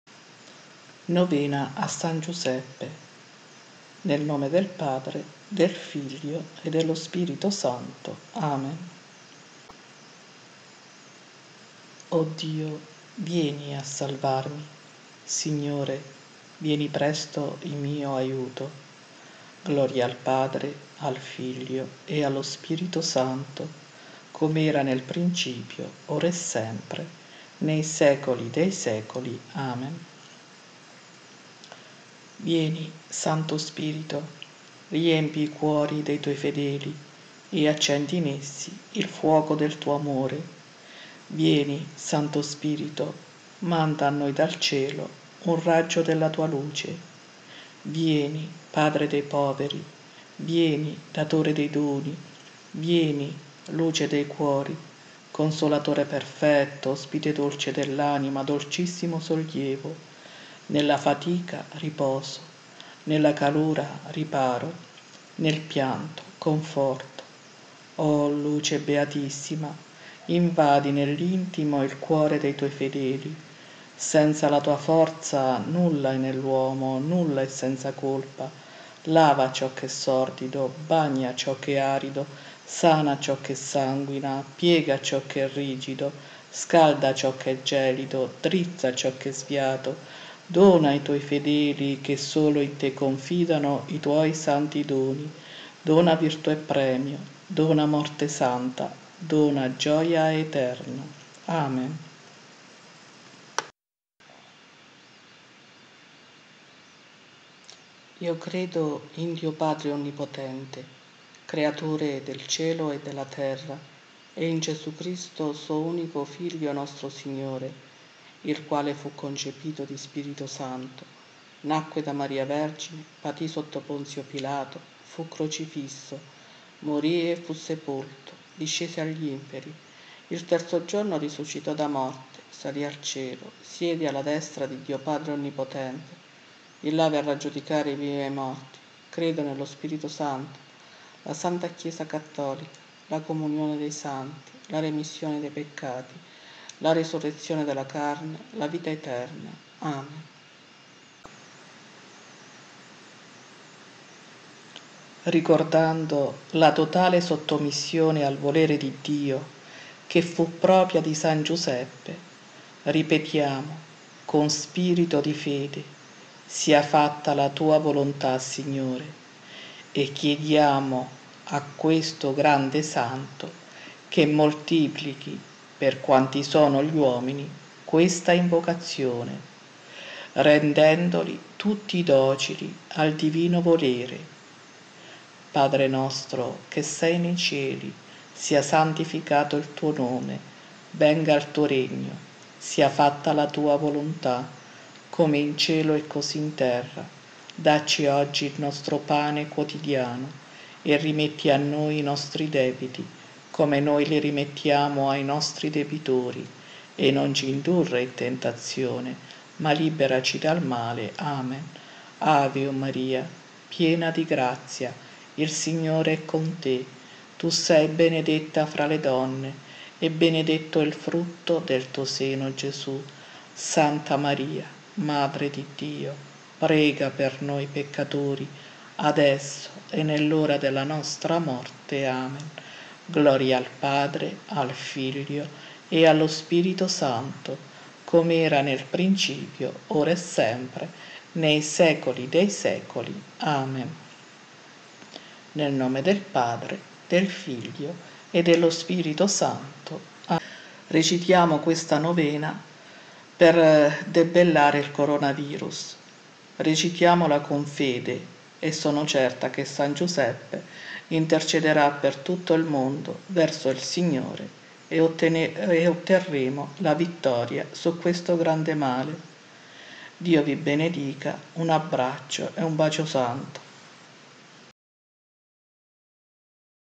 NOVENA A SAN GIUSEPPE 1 GIORNO
O Dio, vieni a salvarmi. Signore, vieni presto in mio aiuto.

Gloria al Padre e al Figlio e allo Spirito Santo. Come era nel principio, ora, e sempre nei secoli dei secoli. Amen.

Sequenza allo Spirito Santo "Vieni, Santo Spirito, riempi i cuori dei tuoi fedeli e accendi in essi il fuoco del tuo amore". Vieni, Santo Spirito, manda a noi dal cielo un raggio della tua luce. Vieni, padre dei poveri, vieni, datore dei doni, vieni, luce dei cuori. Consolatore perfetto, ospite dolce dell'anima, dolcissimo sollievo. Nella fatica, riposo, nella calura, riparo, nel pianto, conforto. O luce beatissima, invadi nell'intimo il cuore dei tuoi fedeli. Senza la tua forza, nulla è nell'uomo, nulla senza colpa. Lava ciò che è sordido, bagna ciò che è arido, sana ciò che sanguina. Piega ciò che è rigido, scalda ciò che è gelido, drizza ciò che è sviato. Dona ai tuoi fedeli che solo in te confidano i tuoi santi doni. Dona virtù e premio, dona morte santa, dona gioia eterna. Amen.

Credo in Dio, Padre onnipotente, creatore del cielo e della terra; e in Gesù Cristo, suo unico Figlio, nostro Signore, il quale fu concepito di Spirito Santo, nacque da Maria Vergine, patì sotto Ponzio Pilato, fu crocifisso, morì e fu sepolto; discese agli inferi; il terzo giorno risuscitò da morte; salì al cielo, siede alla destra di Dio, Padre onnipotente; di là verrà a giudicare i vivi e i morti. Credo nello Spirito Santo, la santa Chiesa cattolica, la comunione dei santi, la remissione dei peccati, la risurrezione della carne, la vita eterna. Amen.

1° giorno

Ricordando la totale sottomissione al volere di Dio, che fu propria di San Giuseppe, ripetiamo con spirito di fede: "Sia fatta la tua volontà, Signore!", e chiediamo a questo grande santo che moltiplichi, per quanti sono gli uomini, questa invocazione, rendendoli tutti docili al divino valore. Pater, Ave, Gloria.

Tratto da: "Preghiamo San Giuseppe" Compagnia amici di Gesù.